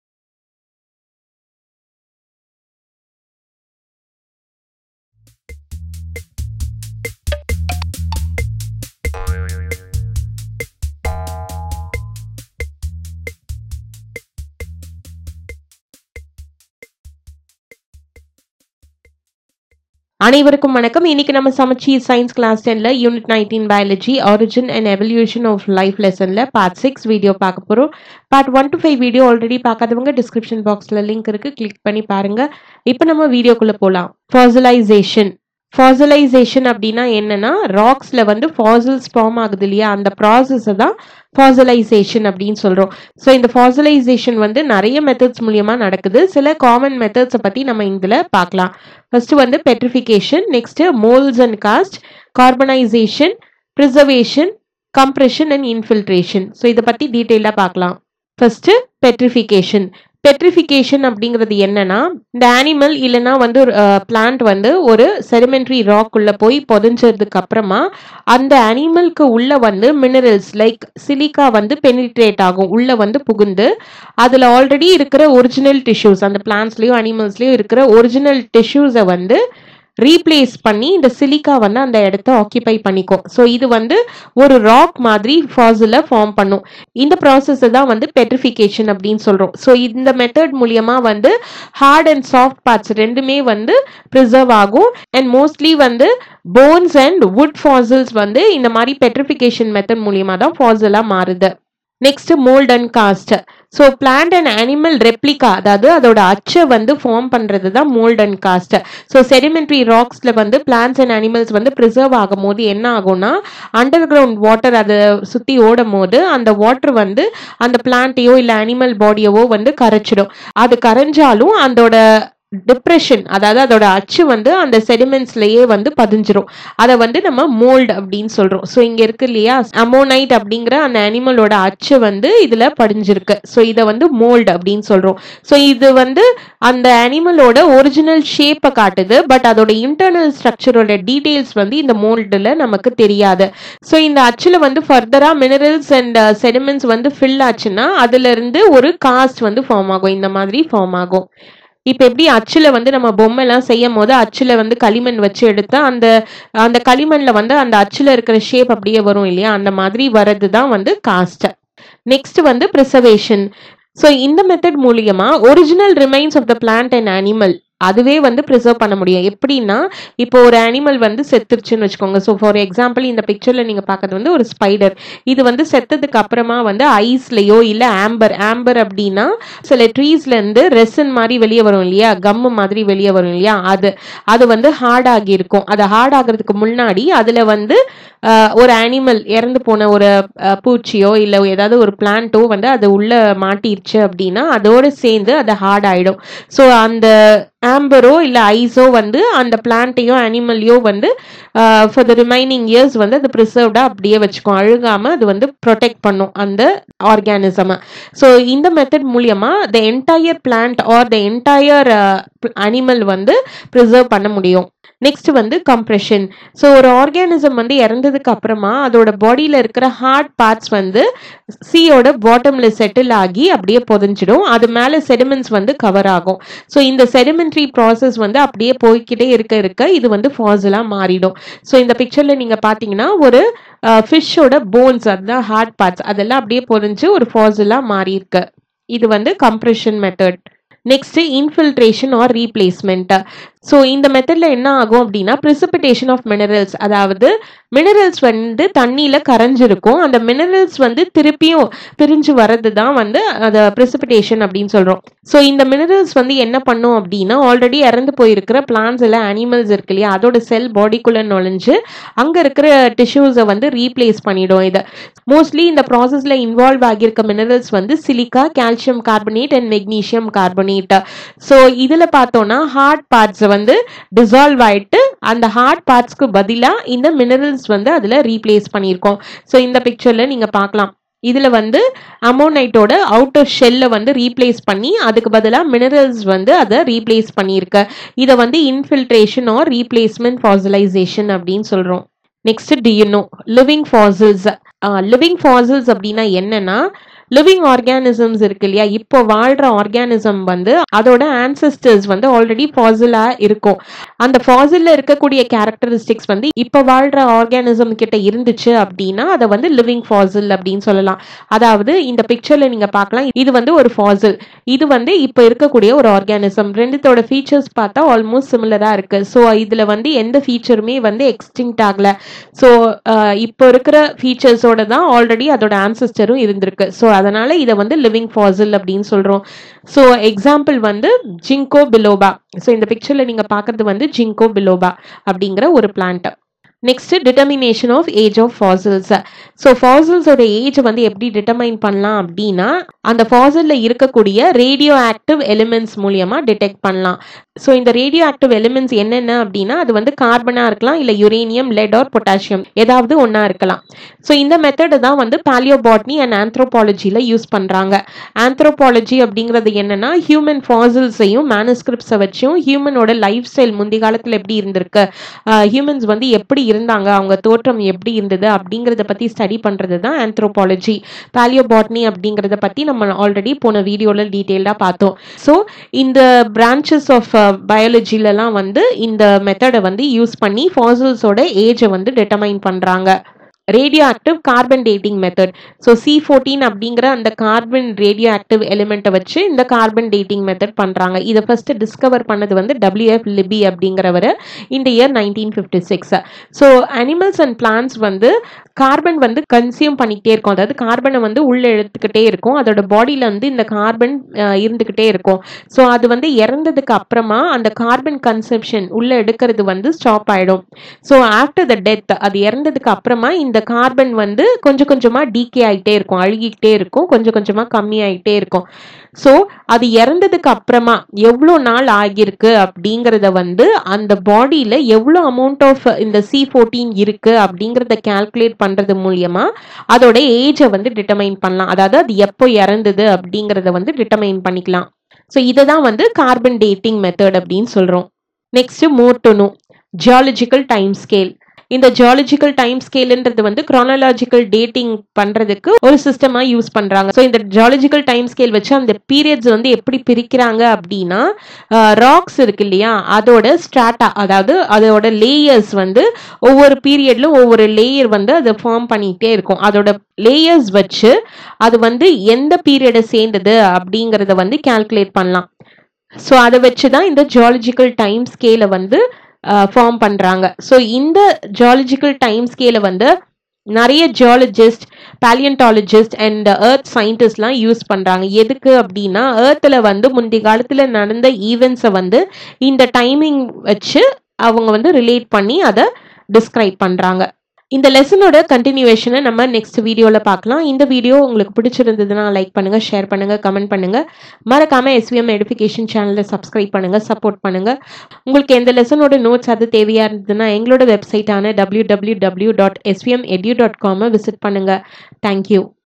SAMACHEER CLASS 10 SCIENCE UNIT 19 ORIGIN & EVOLUTION OF LIFE PART 6 | EXPLANATION IN TAMIL
Unit 19 Origin and Evolution of Life

Introduction, Origin of Earth, Big Bang Theory, Theories on Origin of Life – Special Creation, Spontaneous generation (Abiogenesis), Biogenesis, Extra Terrestrial or Cosmic origin, Chemical Evolution of Life.
Evidences of Evolution, Evidences from Morphology & Anatomy – Homologous organs, Analogous organs, Vestigial organs, Atavism,
Evidences from Embryology – Biogenetic law or Recapitulation theory
Evidences from Palaeontology, Archaeopteryx
Theories of Evolution – Lamarckism, Principles of Lamarckism – Internal Vital Force, Environment & new needs, Use & disuse theory, Theory of inheritance of acquired characters.
Darwinism or Theory of Natural Selection – Principles of Darwinism – Over production, Struggle for Existence (Intra Specific, Inter Specific, Environmental), Variations, Survival of the Fittest or Natural Selection, Origin of Species
Variation – Types – Somatic & Germinal Variation (Continuous & Discontinuous Variation)
Relationship between Mutation & Variation, Paleobotany, Importance of fossils, Kaspar Maria Von Sternberg, Birbal Sahani, Fossilization (Petrification, Mold & Cast, Preservation, Compression, Infilteration or Replacement)
Determination of age of fossils, Radioactive Carbon dating method, Ethnobotany, Aspects of Ethnobotany, Importance of Ethnobotany, Astrobiology or Exobiology,